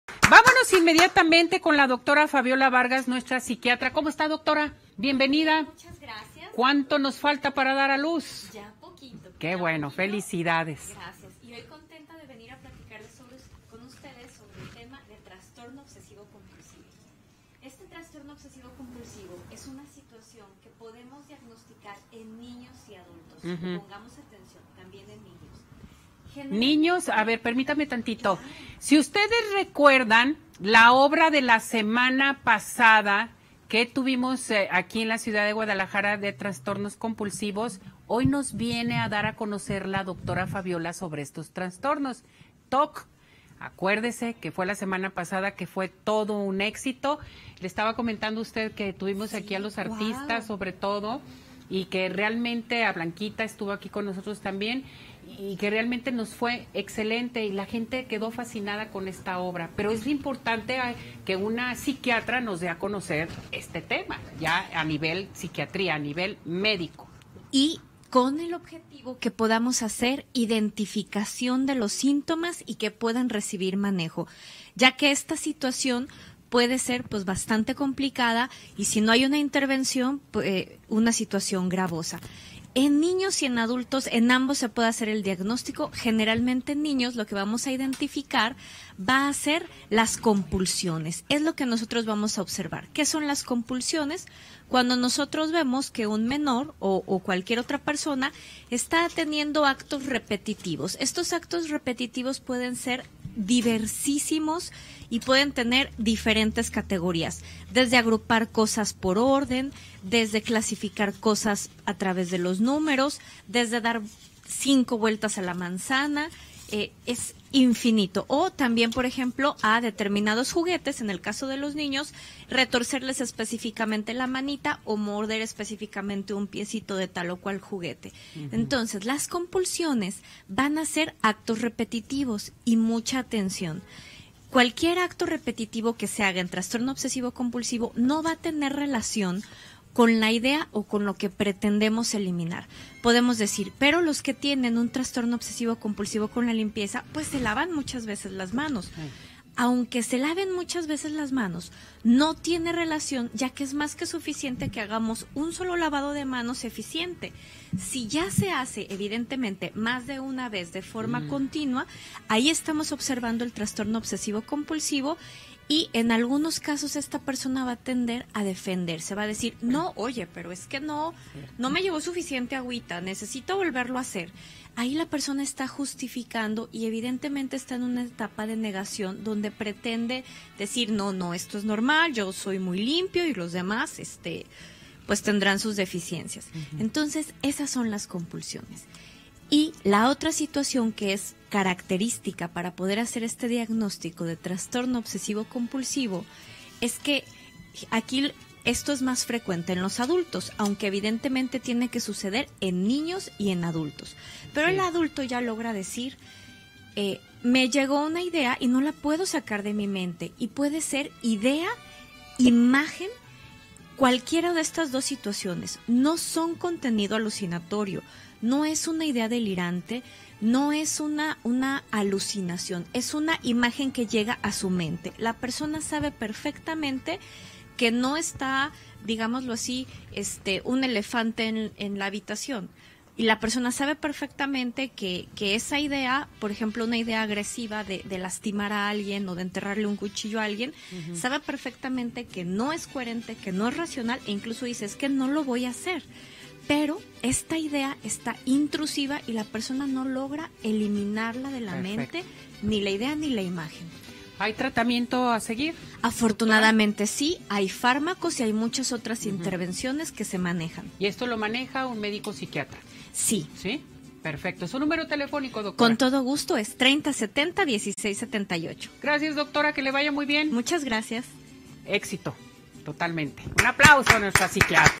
Psiquiatra – TOC
¿Cómo podemos detectar el trastorno obsesivo compulsivo TOC?
Nos explica la Dra. Fabiola Vargas, Psiquiatra.